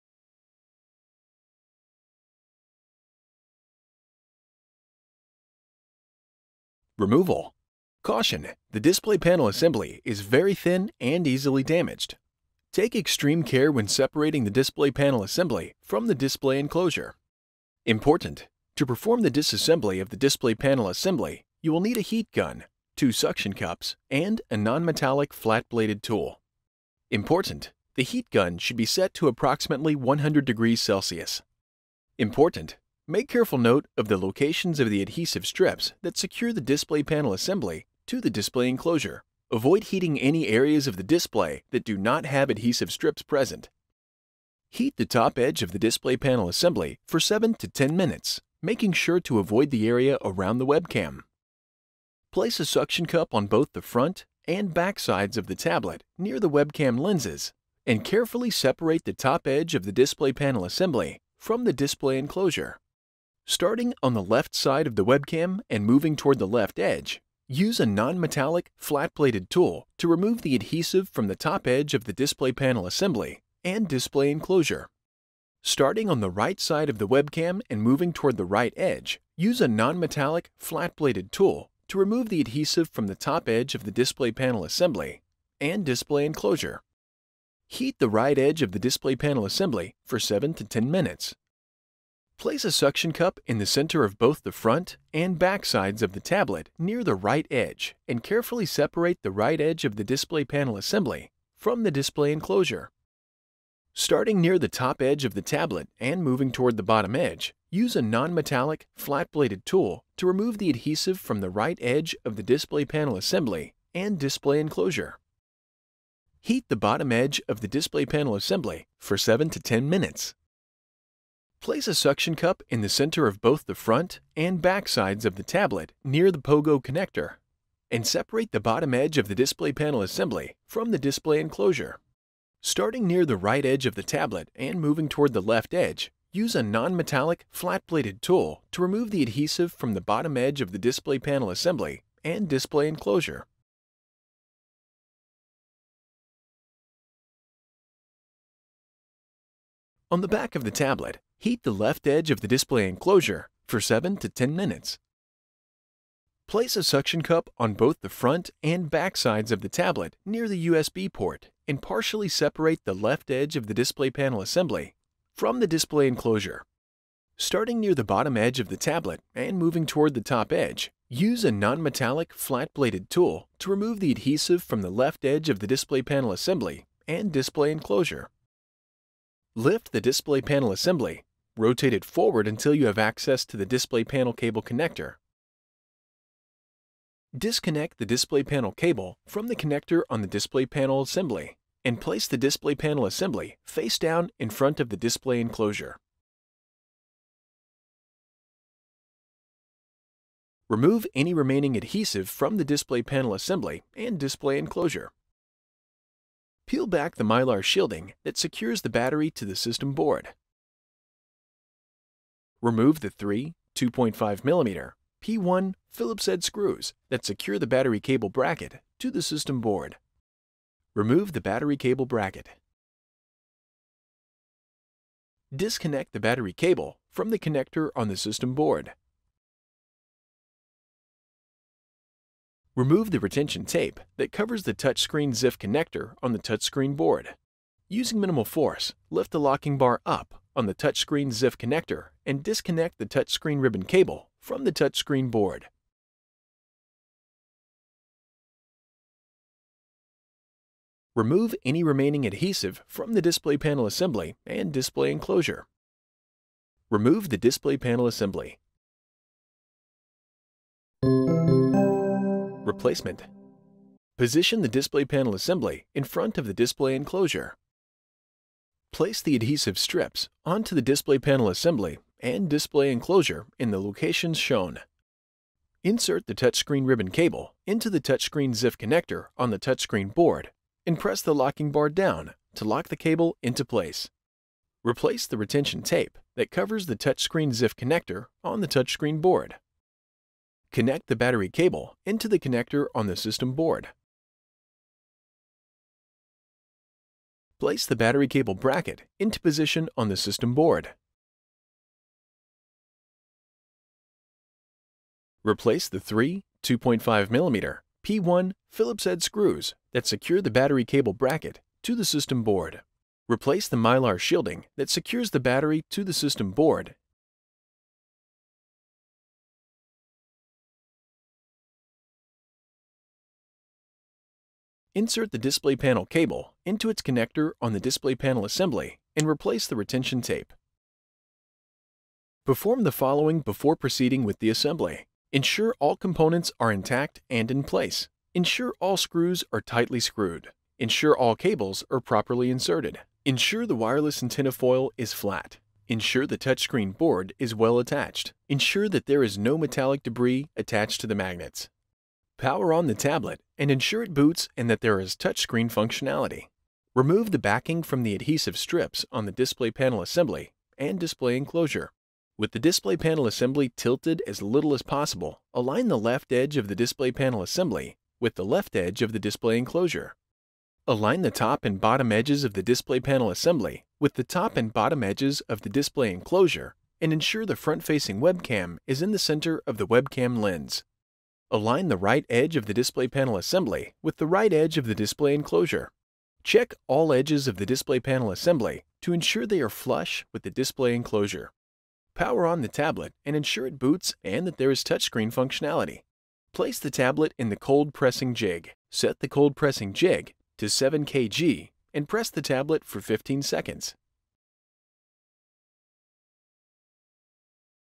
Remove and Replace the Display Panel Assembly | HP ENVY x2
Learn how to remove and replace the display panel assembly on the HP ENVY x2.